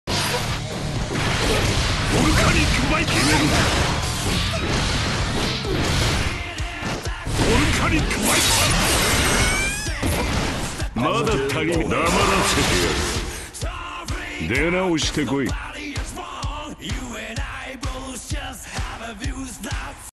Patch Notes:"Sol's Nerfed But His Crouch Slash is Buffed" Me:"Its Free Real Estate"
Daisukes Vision Never Felt So Good.